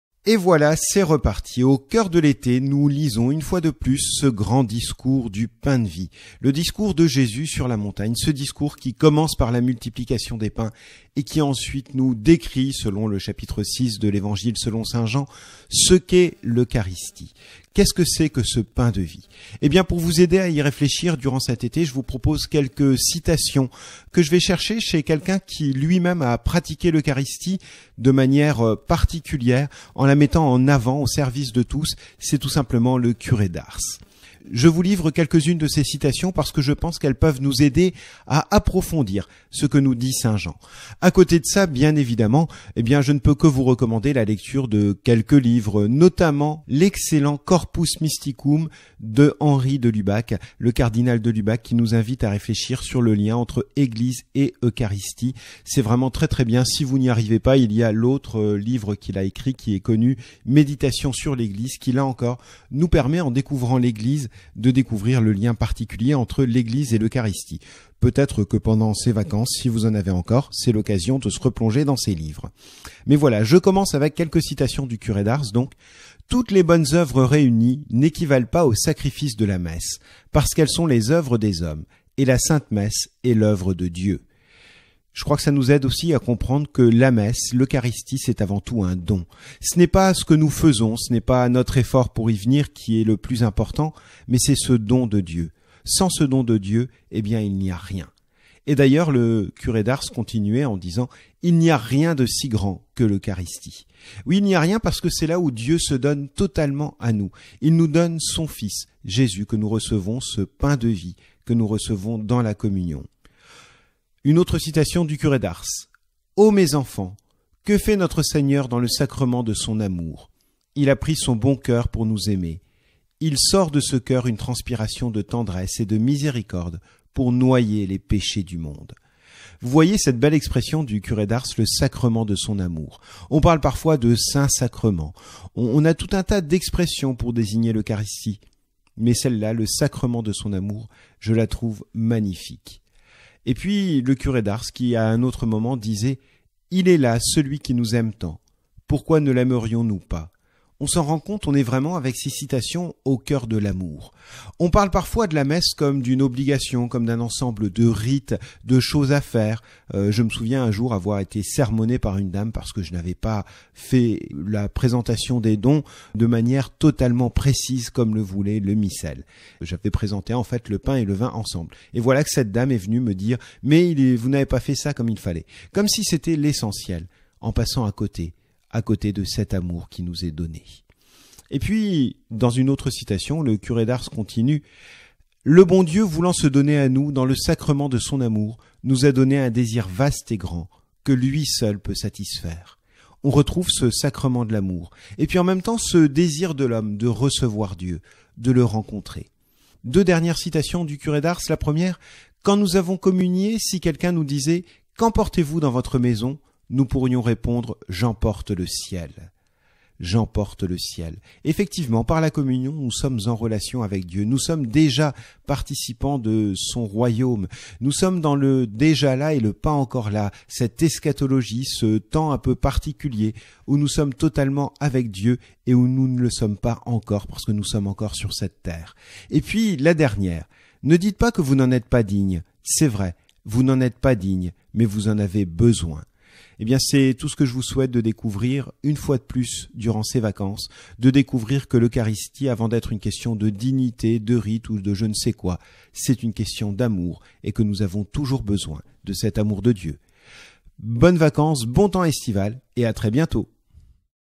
Eucharistie et Pain de Vie
L'été est la période où l'Eglise lit le discours sur le pain de vie... Mais au delà de ce qu'on sait, de ce qu'on imagine, si l'on redécouvrait ce texte de saint Jean ? P. Lemessin.